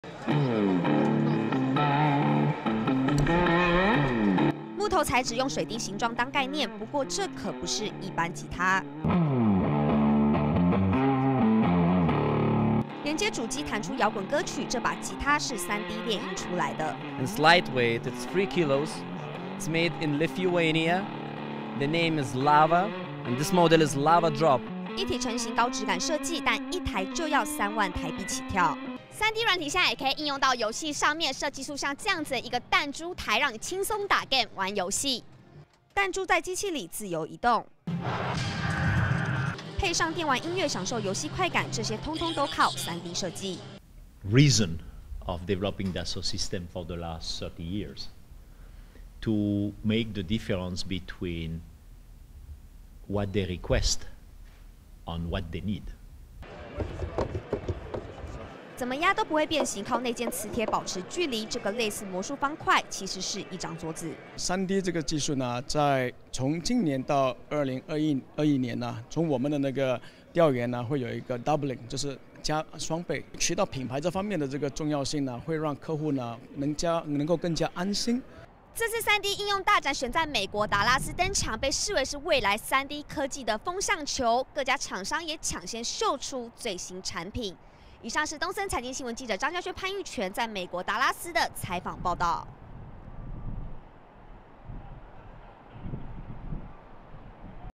直擊3D大展!木製吉他.3D設計彈珠台
3D列印現在應用面更廣，可以用到遊戲上!在美國3D大展上，就有一台彈珠台，全都用3D軟體設計，還可以結合音樂玩遊戲！另外還有一把木製吉他，特殊質感水滴形狀，全都靠3D列印，不過一台就要價3萬台幣，來看我們來自美國的第一手報導!